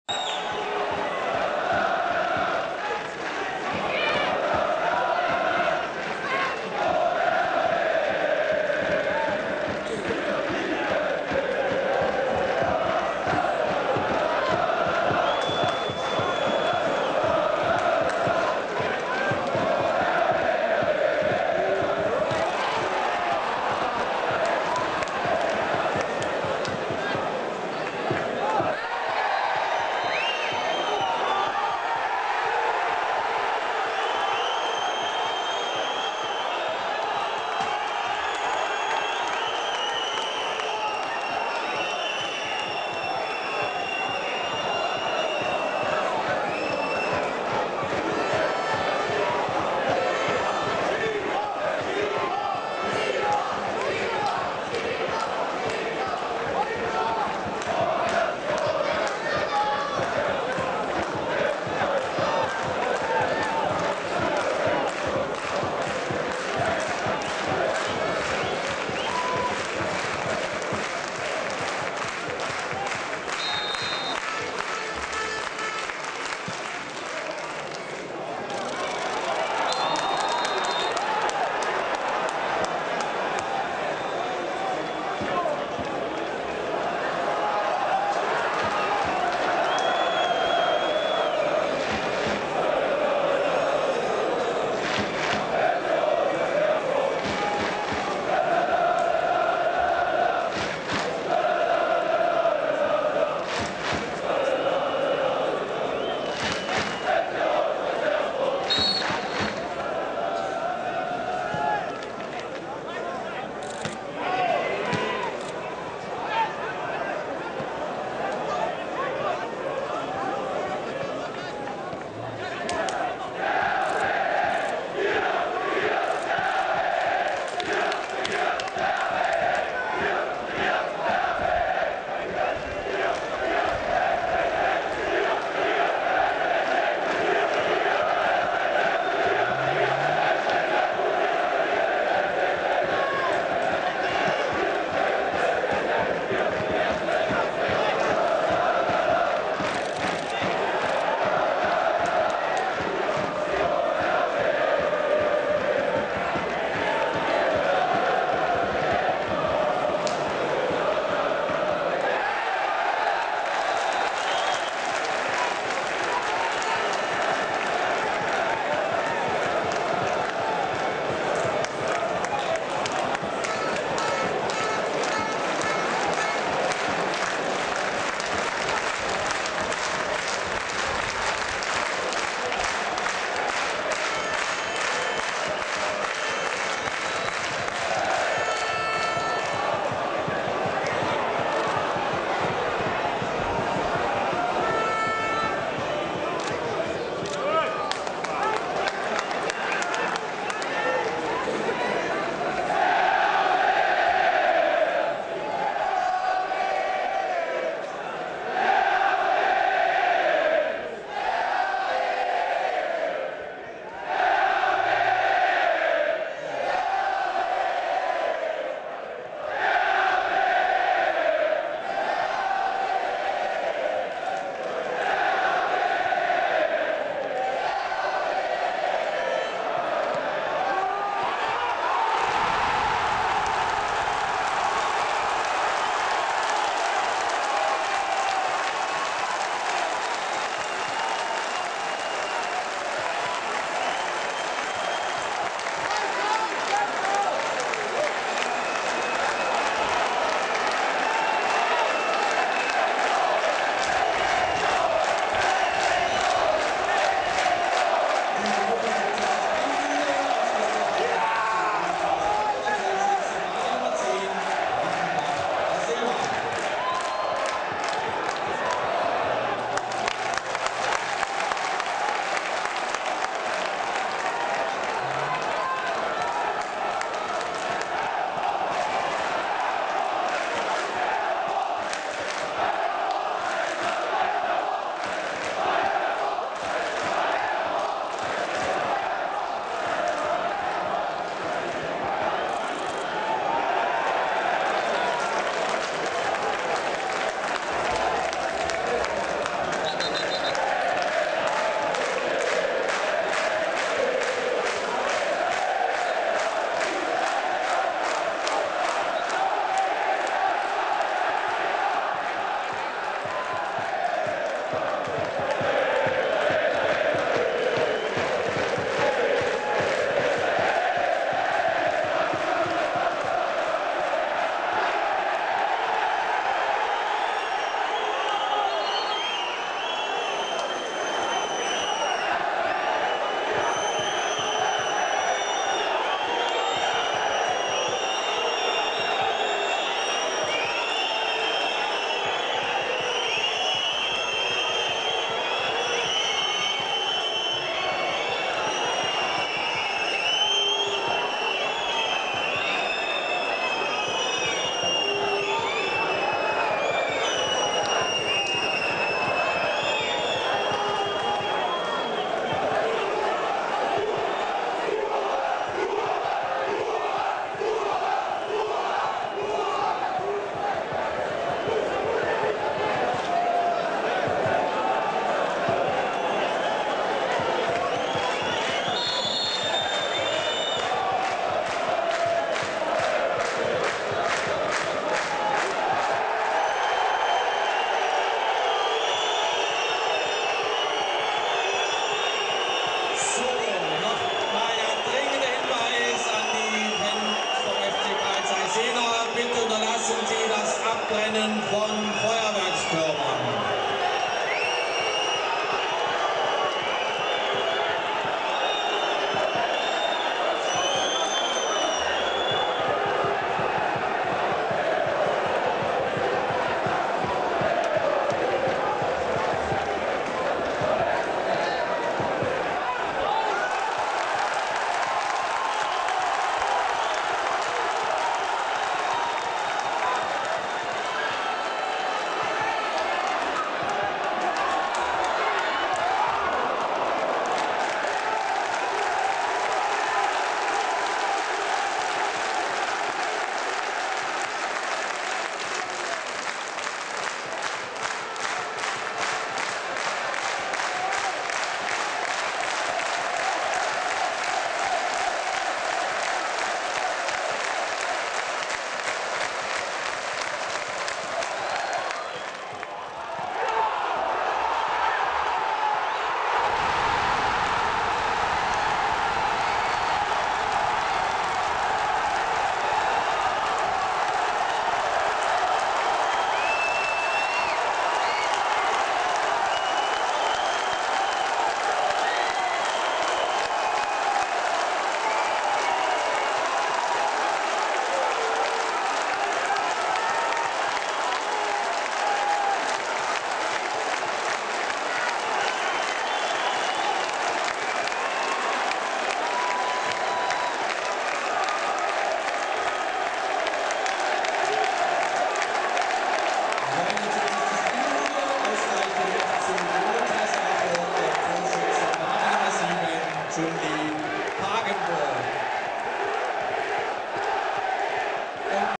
FC Carl-Zeiss Jena vs FC Rot-Weiß Erfurt Thüringen-Pokal 2009 - Die Entscheidung 1/3
Rot-Weiß-Erfurt holt innerhalb von 4 Minuten ein 2:0 gegen J*** auf und siegt am Ende mit 2:3... Alle drei Tore in 13 Minuten. Hier die entscheidenden letzten 20 Minuten des Spiels ungeschnitten!!